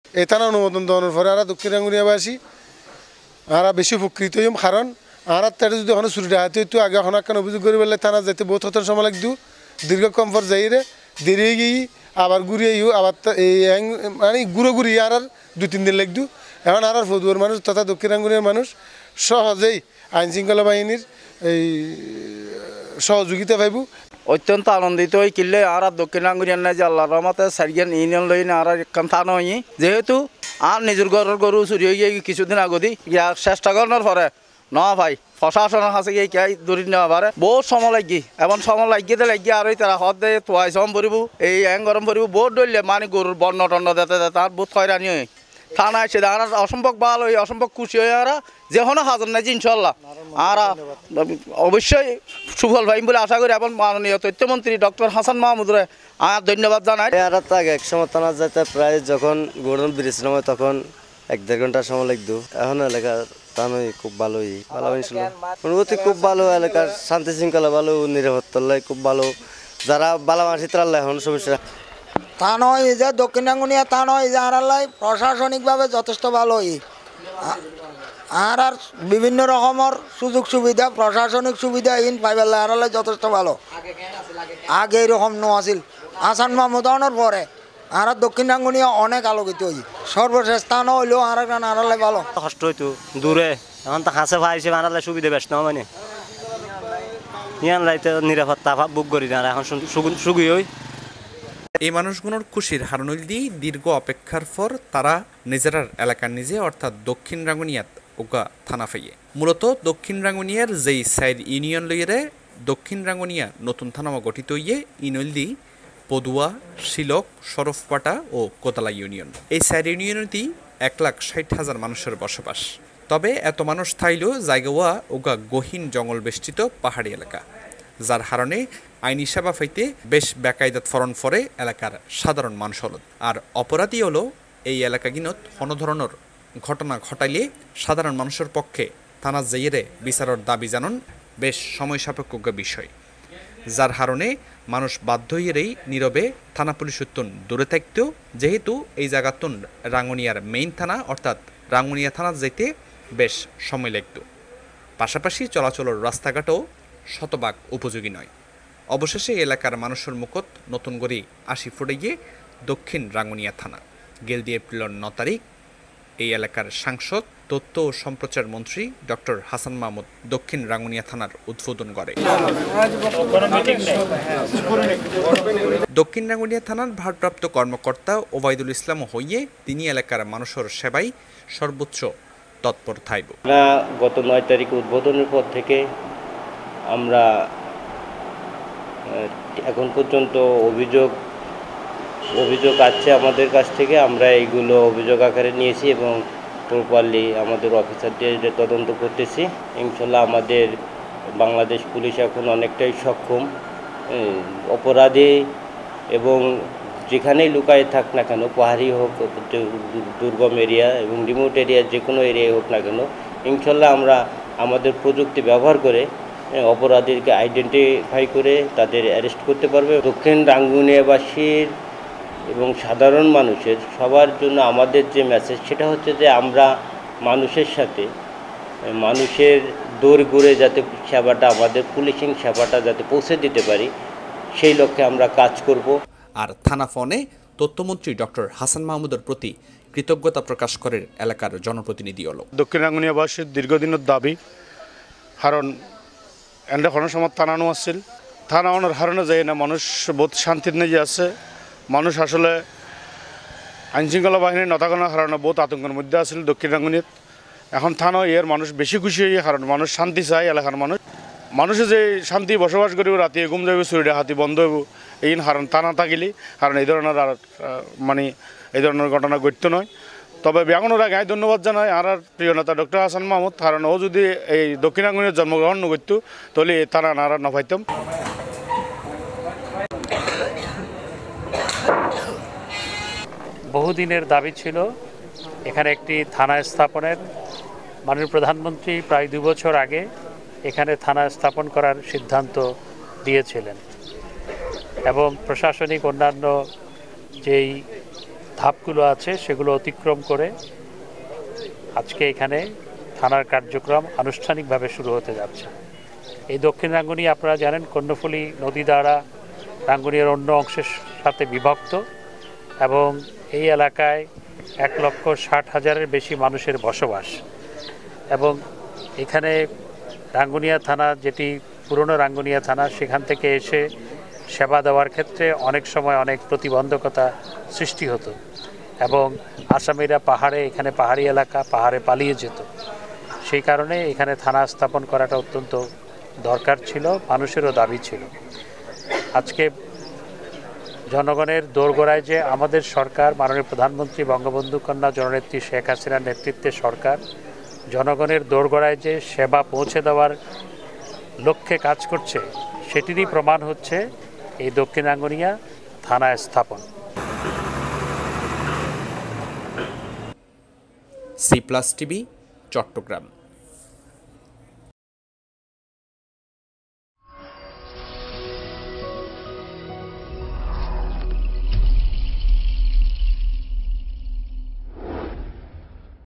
৪ ইউনিয়ন নিয়ে রাঙ্গুনিয়া নতুন থানা,উচ্ছ্বসিত এলাকাবাসী | Cplus
গহীন জঙ্গল থেকেও আসামি ধরে আনবে নতুন থানা পুলিশ। বহুদিন পর ৪ ইউনিয়ন নিয়ে দক্ষিণ রাঙ্গুনিয়া নতুন থানা পাওয়ায় আত্মবিশ্বাস বেড়েছে, উচ্ছ্বসিত এলাকাবাসী।
আপনার পছন্দের অনুষ্ঠানগুলো দেখুন নিয়মিত
Slogan: আরাঁ চাটগাঁইয়া হতা হই
Country: Bangladesh
Language: Bengali।Chittagong Language
Broadcast area: Online